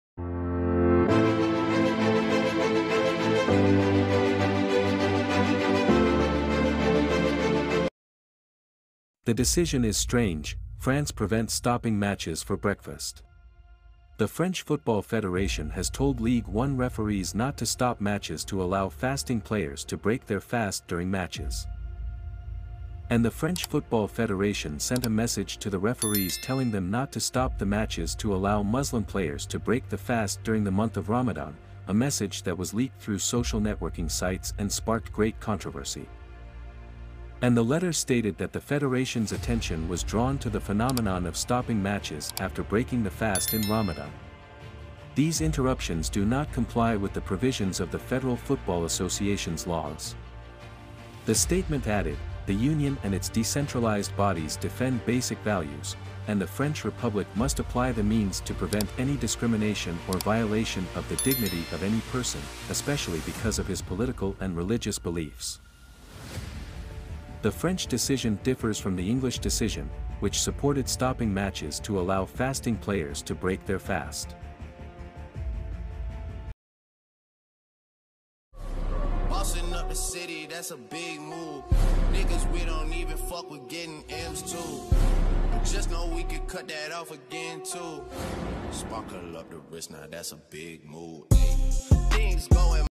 The decision is strong France prevents stopping matches for breakfast
The decision is strange, France prevents stopping matches for breakfast
The French Football Federation has told Ligue 1 referees not to stop matches to allow fasting players to break their fast during matches
And the French Football Federation sent a message to the referees telling them not to stop the matches to allow Muslim players to break the fast during the month of Ramadan, a message that was leaked through social networking sites and sparked great controversy.
And the letter stated that the Federation’s attention was drawn to the phenomenon of stopping matches after breaking the fast in Ramadan. These interruptions do not comply with the provisions of the Federal Football Association’s laws.
The statement added, “The Union and its decentralized bodies defend basic values, and the French Republic must apply the means to prevent any discrimination or violation of the dignity of any person, especially because of his political and religious beliefs.”
The French decision differs from the English decision, which supported stopping matches to allow fasting players to break their fast